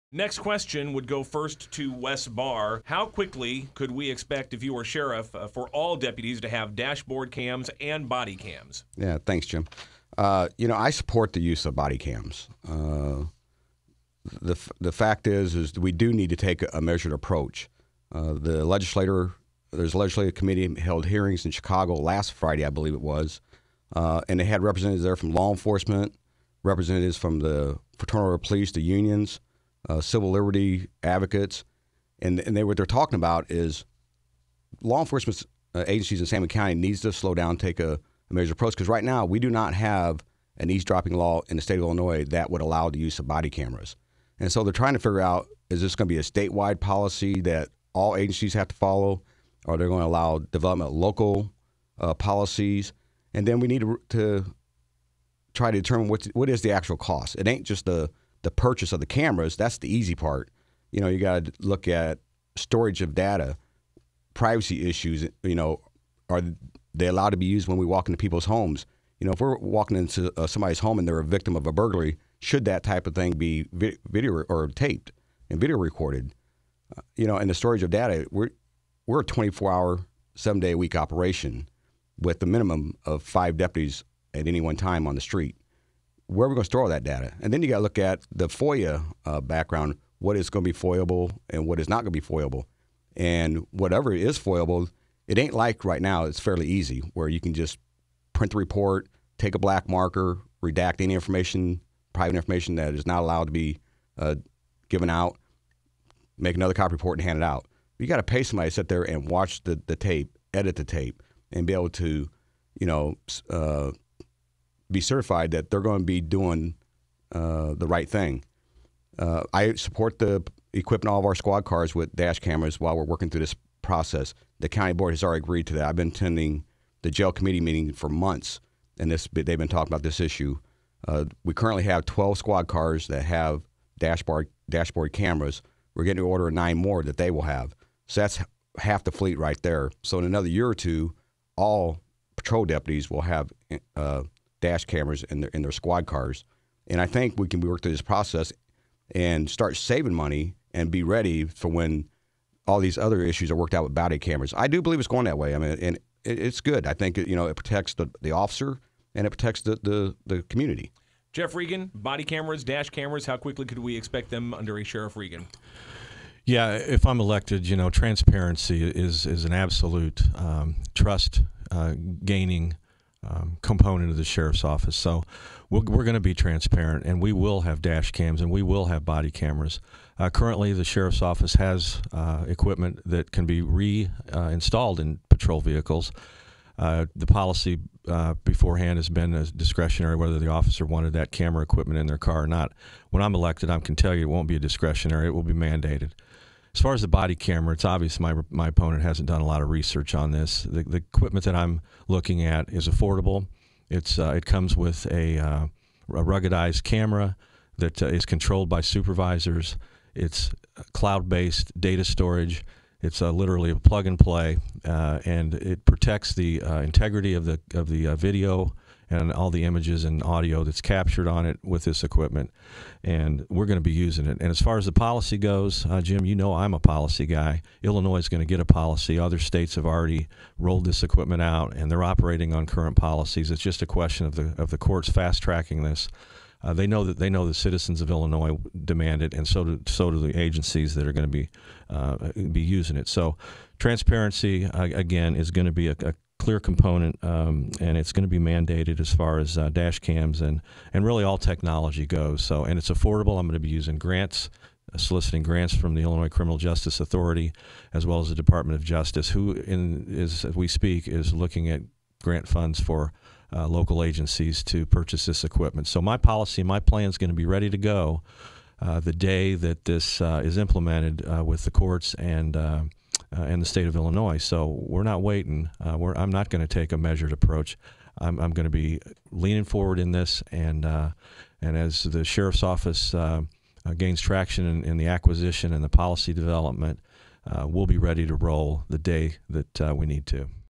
Sangamon County Sheriff Debate - Dashboard Cameras
Jim Leach asks Sangamon County Sheriff candidates when can the voters expect all deputies to have dashboard and body cameras.

Listen to the complete debate online at WMAY.com